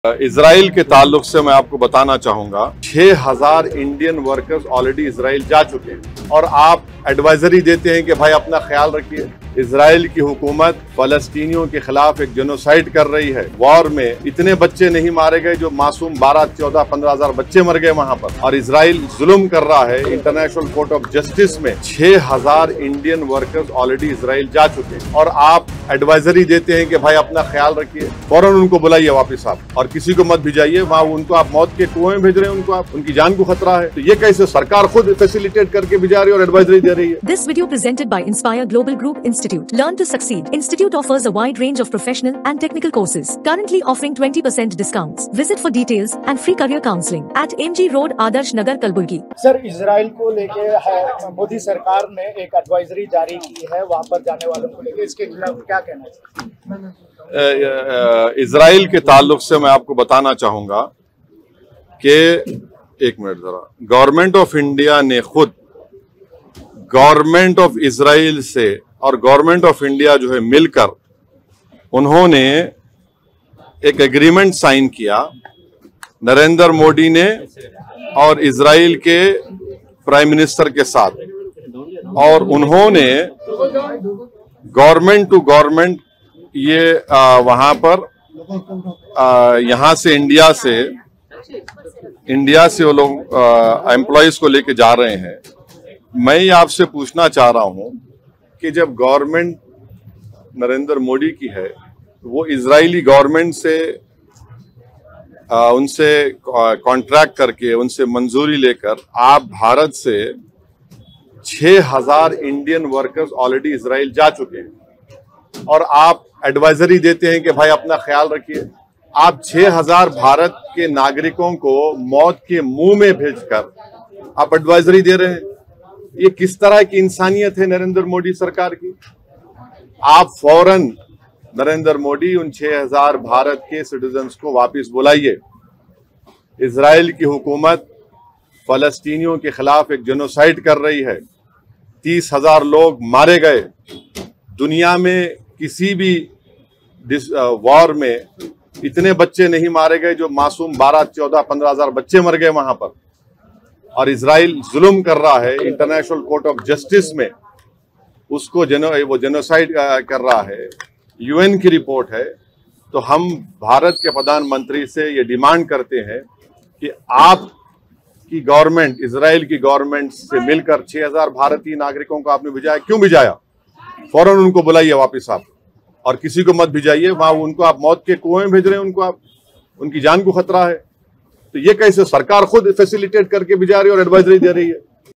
Modi Sarkar Ne 6000 Bhartiya Nagrikoon Ko Israil Kiu Bheja? Asad Owaisi
Modi Sarkar Ne 6000 Bhartiya Nagrikoon Ko Israil Kiu Bheja? Asad Owaisi Ne ki Jald se Jald Wapas Lane ki Mang